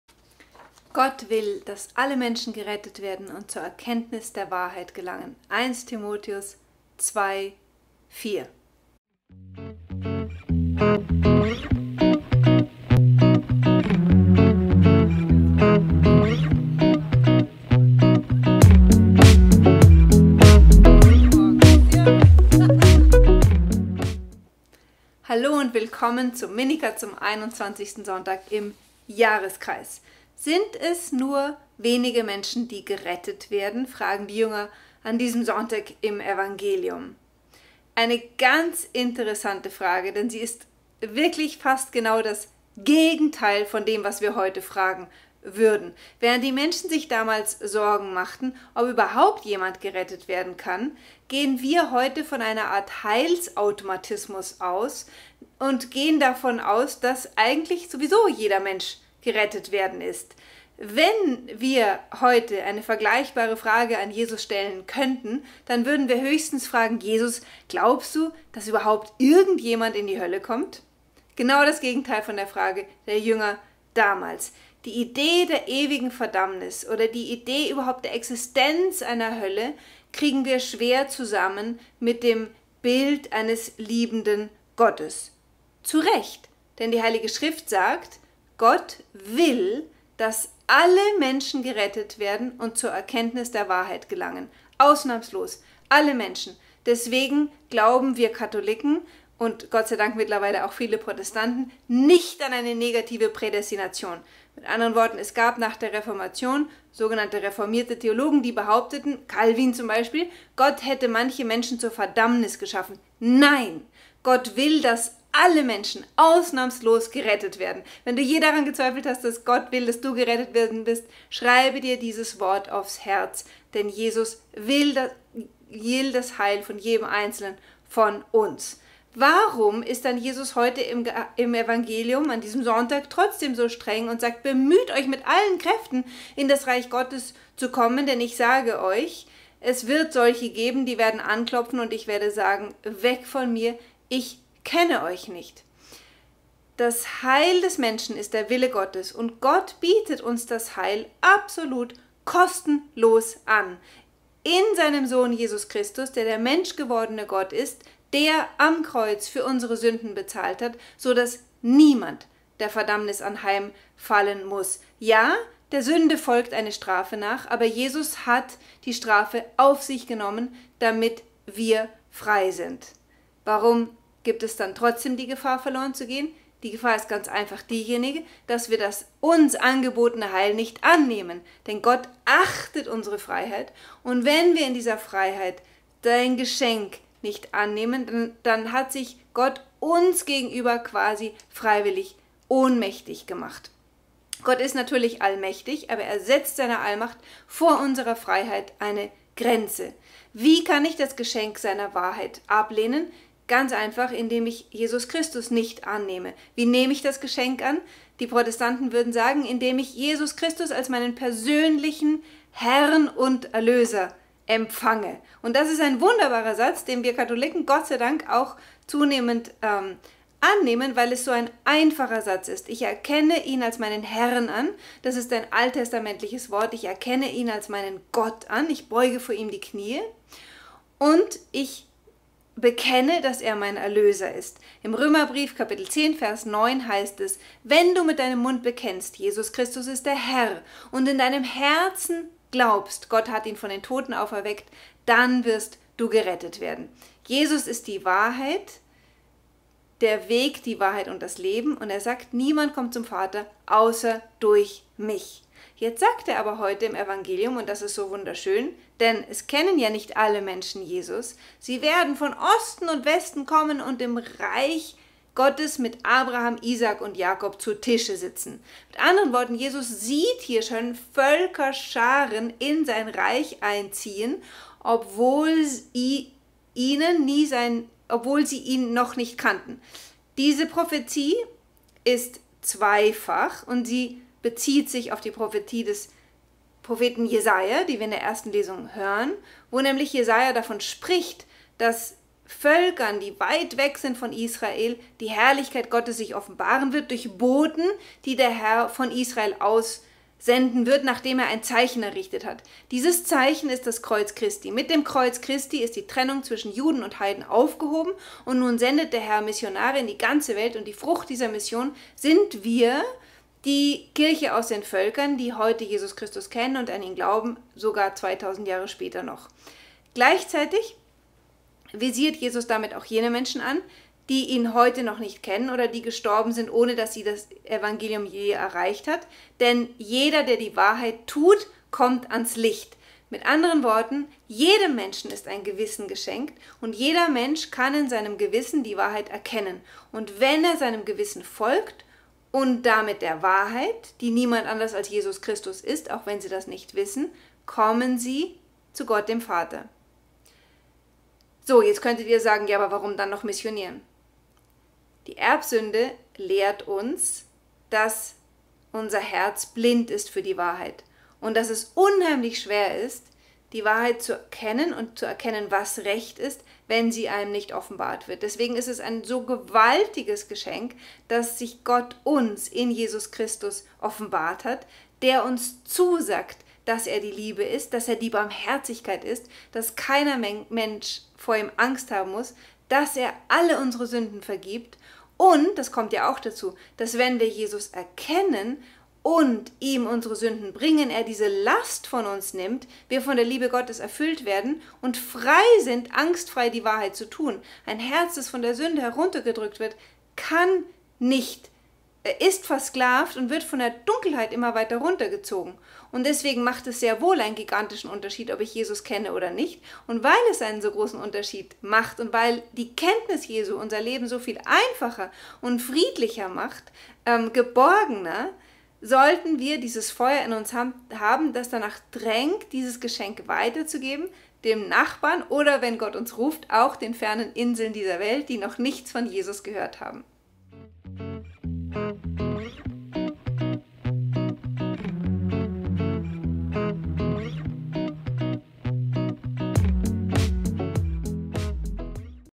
21. Sonntag im Jahreskreis (C): Gott will, dass alle Menschen gerettet werden (1 Tim 2:4)
Minikatechese zu den Lesungen, Jes 66:18-21; Lukas 13:22-30